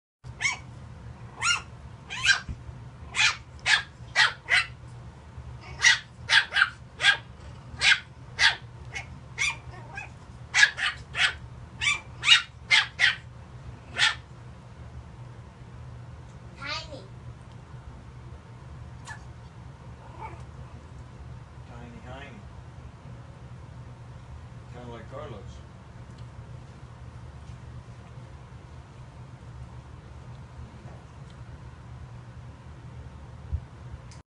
Cute tiny puppy barks at cat
A cute puppy dog barks at a sitting cat indoors.